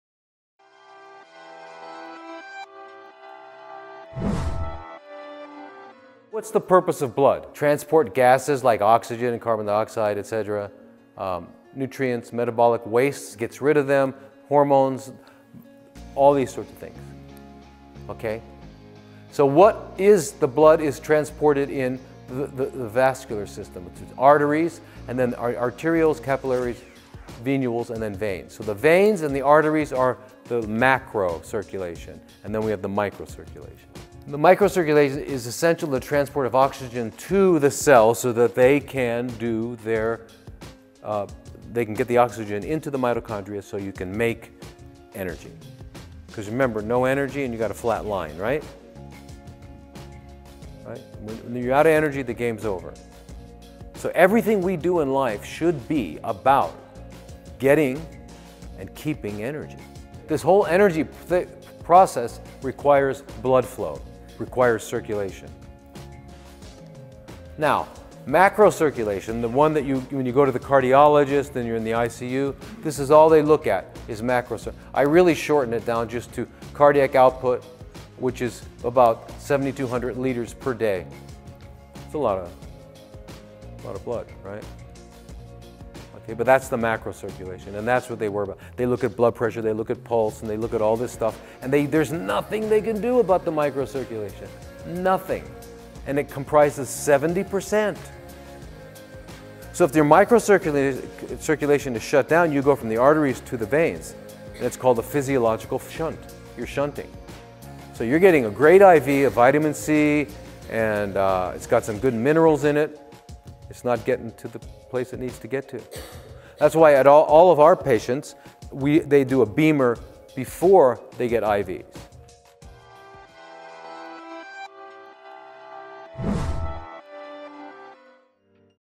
What Is The Purpose Of Blood?
What is the purpose of blood? Blood has many functions and roles including the transport of gases like oxygen and carbon dioxide. It also transports nutrients and helps get rid of metabolic wastes.

How is the blood transported in the vascular system? It is transported through veins, arteries, capillaries, and arterioles. The veins and the arteries are the macrocirculation.

Microcirculation is essentially the transport of oxygen into the cell to the mitochondria so that energy can be made. Remember, if there is no energy then you have a flat line.

When a person runs out of energy the game of life is over. Everything we do in life should be about getting and keeping energy. This whole energy process requires blood flow. It requires circulation.

Macrocirculation is all that a cardiologist will look at when you’re in the ICU. The most important part is the cardiac output which is approximately 7,200 liters per day. That’s a lot of blood, right?

That’s macrocirculation. They look at blood pressure, your pulse, etc. However, with microcirculation, there’s nothing they can do, and it comprises seventy percent.

 So, if your microcirculation is shut down, you go from the arteries to the veins. That’s called the physiological shunt. You’re shunting. In this case, even if you are getting a great iv of vitamin c, the minerals in it will not get to the place it needs to get to.

That’s why all of our patients will do a BEMER before getting an IV. The BEMER helps with the microcirculation and therefore helps with the absorption of the nutrients and minerals from the IV.

Dr. Thomas Lodi
Institute of Integrative Oncology
51 Goldhill Plaza #07-10/11
Singapore 308900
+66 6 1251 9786